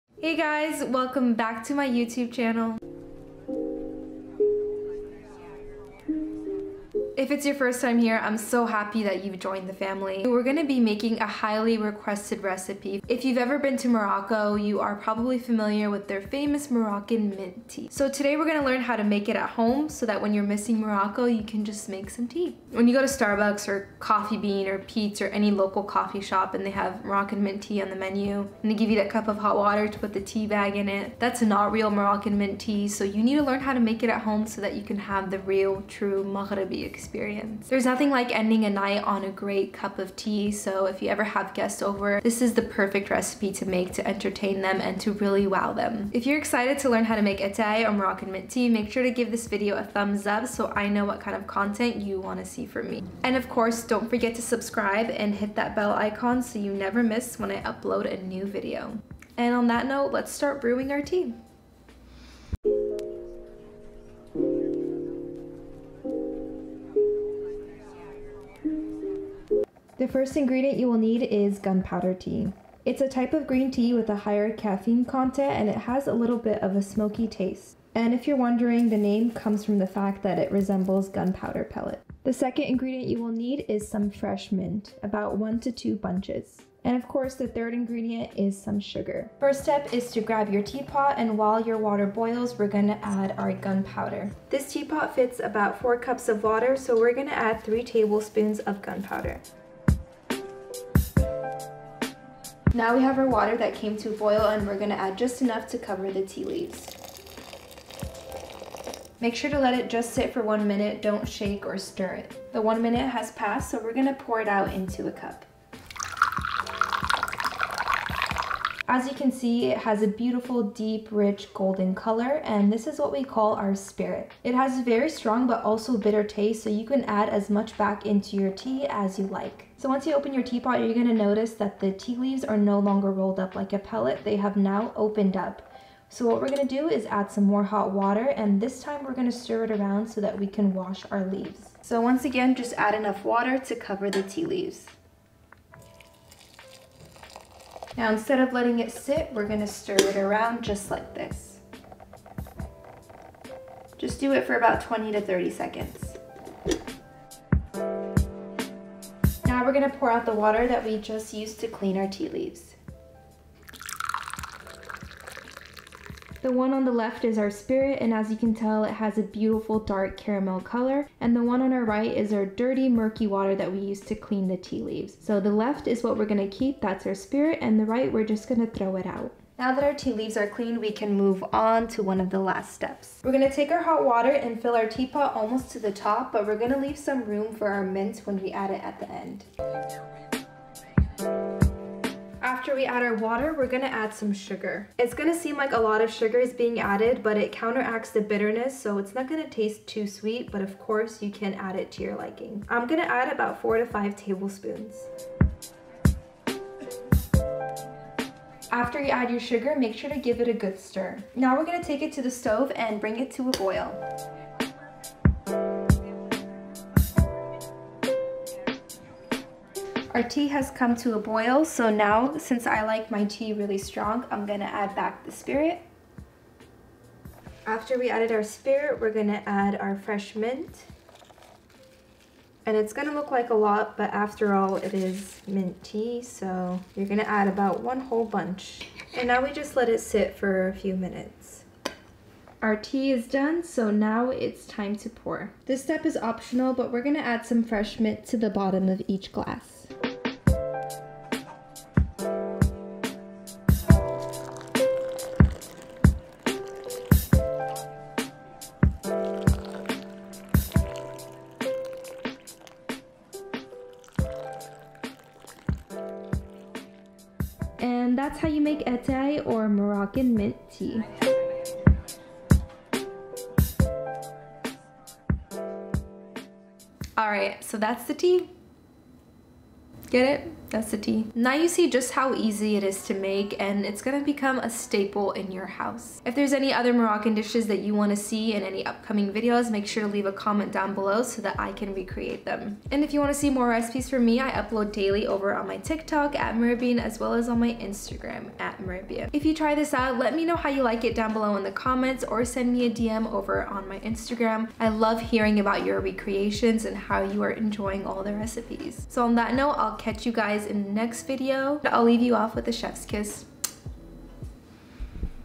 How to Make Moroccan Mint Tea - Atay Tutorial
If you have ever traveled to Morocco, you are definitely obsessed with their famous green tea with mint or as they call it, Atay! Now you can make this North African staple at home with just 4 simple ingredients.